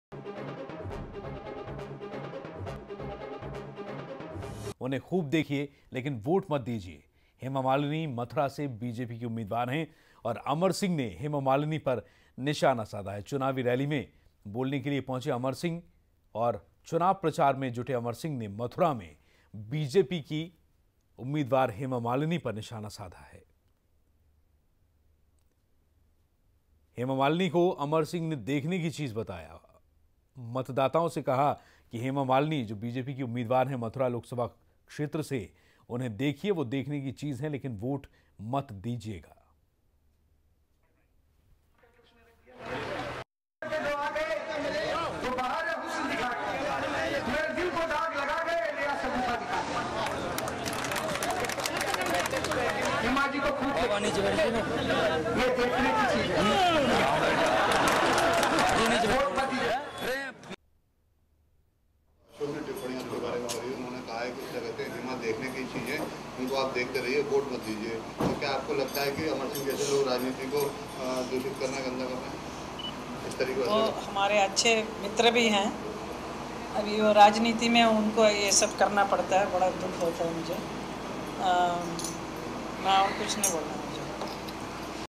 Hema Malini dekhne ki cheez, Don't vote her : Amar Singh
Hema Malini dekhni wali cheez, khoob dekhiye par Vote na deejiye ( "Such leaders attract people but this attraction does not ransform into voting,") Amar Singh's "Dekhne wali cheez" remark on Hema Malini spark controversy Amar Singh also said Hema Malini's religion is money.